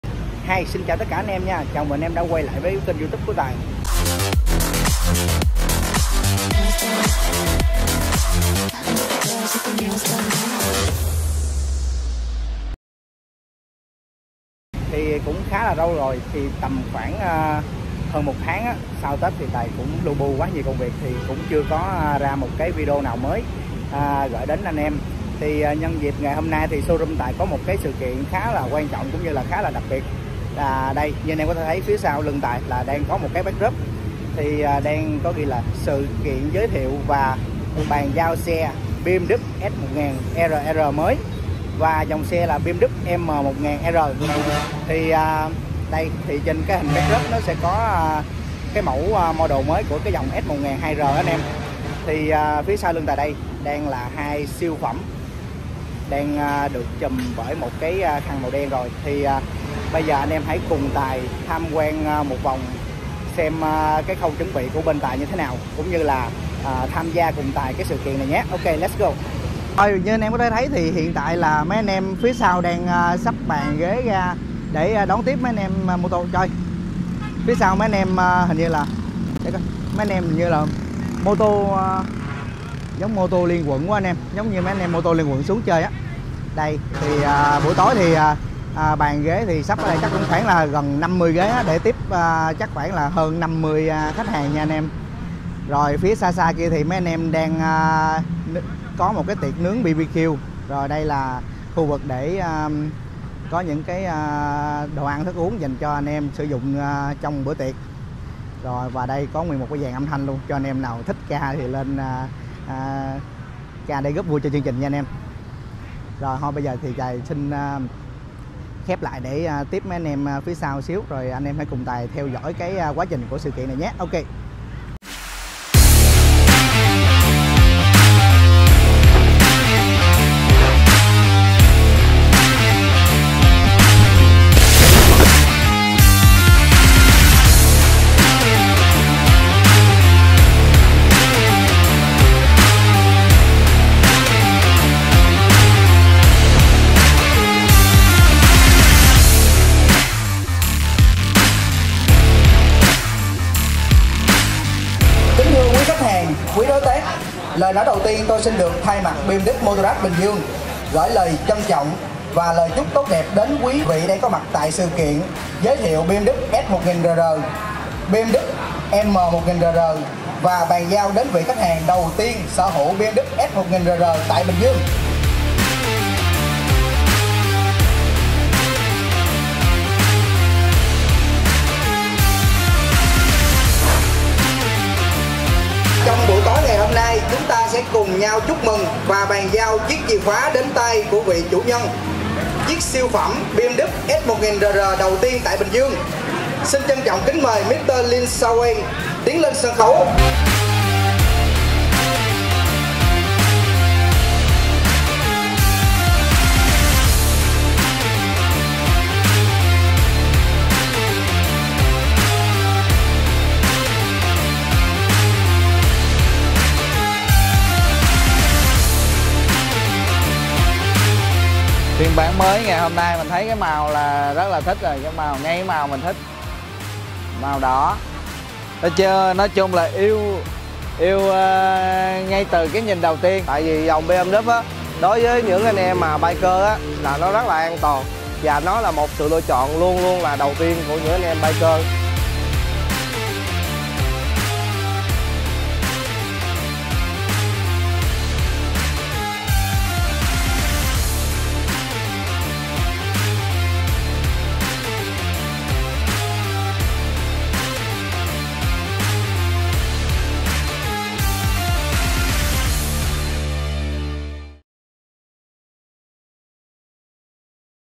Sự kiện ra mắt BMW S1000 RR, M1000 R và Bàn Giao đến vị khách hàng Đầu Tiên tại Bình Dương.
LỜI CẢM ƠN TRI ÂN KHÁCH HÀNG ĐAM MÊ TỐC ĐỘ & SỰ HOÀN HẢO CỦA BMW MOTORRAD

Đội ngũ BMW Motorrad Bình Dương bày tỏ lòng biết ơn sâu sắc đến tất cả quý khách hàng đã tham dự và ủng hộ sự kiện giới thiệu đặc biệt của chúng tôi, BMW M 1000 R và BMW S 1000 RR mới tại Bình Dương. Sự kiện đã thành công ngoài sức mong đợi, và chúng tôi vô cùng hạnh phúc vì được chia sẻ niềm đam mê này cùng với mọi người.

Đặc biệt, chúng tôi muốn gửi lời cảm ơn đặc biệt tới Mr. Lin Shaowei - người đã đặt cọc chiếc BMW S 1000 RR đầu tiên tại Bình Dương từ rất sớm và chúng tôi vinh dự khi tiến hành bàn giao chìa khoá đến Mr. Lin Shaowei trong không khí nhộn nhịp ngày hôm đó. Sự tin tưởng và ủng hộ của quý vị là động lực lớn giúp chúng tôi không ngừng nâng cao chất lượng dịch vụ và sản phẩm.

Chiếc BMW S 1000 RR mới không chỉ là một phương tiện di chuyển, mà còn là biểu tượng của sự đẳng cấp, phong cách đặc biệt mang những tính năng an toàn cho các biker. Chúng tôi hy vọng rằng chiếc xe sẽ mang lại cho Mr. Lin Shaowei những trải nghiệm đầy mãn nhãn và hạnh phúc trên mỗi chuyến đi.

Hãy tiếp tục theo dõi fanpage của chúng tôi để cập nhật thông tin về các sự kiện và sản phẩm mới nhất từ BMW Motorrad Bình Dương. Một lần nữa, chúng tôi xin chân thành cảm ơn sự ủng hộ và lòng tin yêu của quý vị!

Trân trọng,
BMW Motorrad Bình Dương